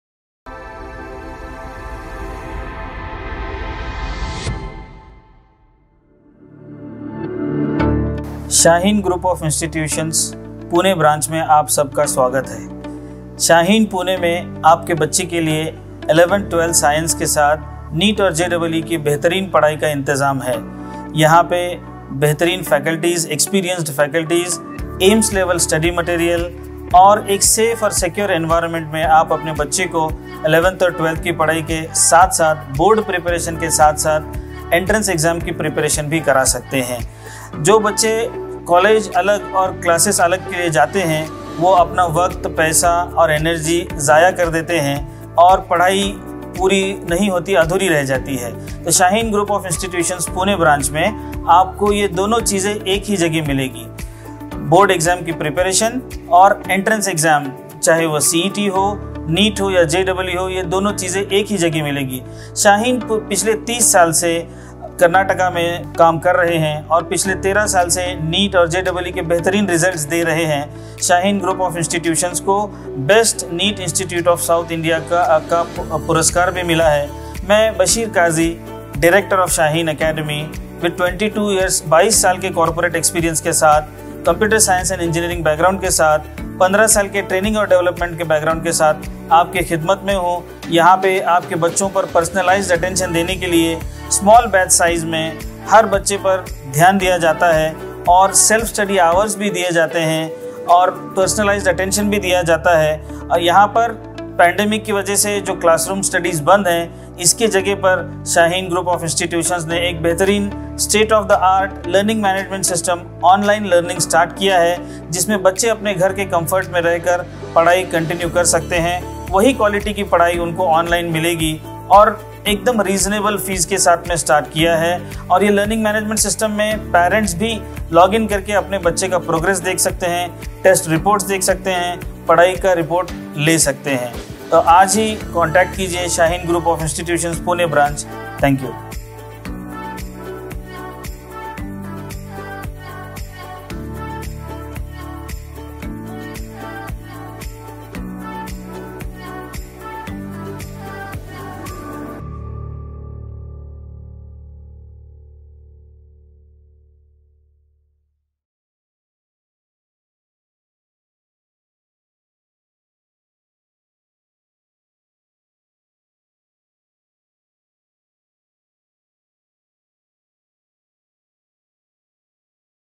1277 Free seats at MBBS in last 7 Years (Bidar)& Merit Students in 12th Science HSc exams at Shaheen
327 Students secured Free seats in Govt. Medical (MBBS) Colleges.
What is Shaheen Pattern ?
What is the Secret of the flying colors of Students learning at Pune branch of Shaheen Group of Institutions.

How Board studies are covered ?

How NEET/JEE is taught with best of class infrastructure, Faculty team and AIIMS level study material?

Get all the answers in the above video.